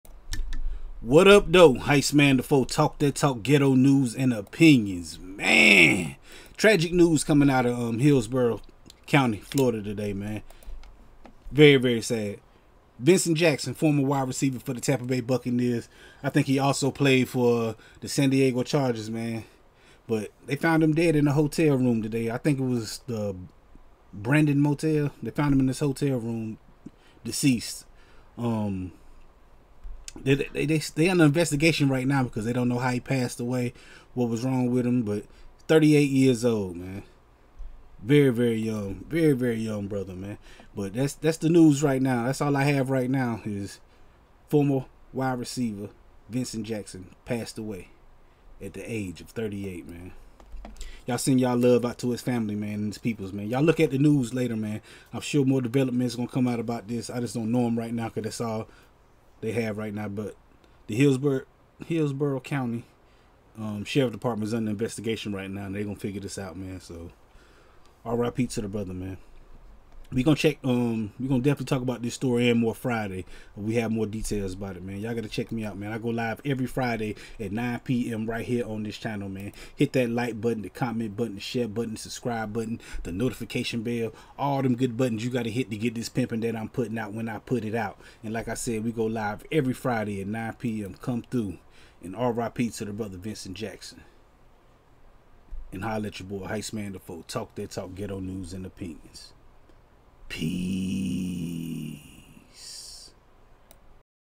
Vincent Jackson DEAD at age 38...found in Brandon Hotel...Cops INVESTIGATING
Former Tampa Bay Wide Receiver Vincent Jackson passed away at 38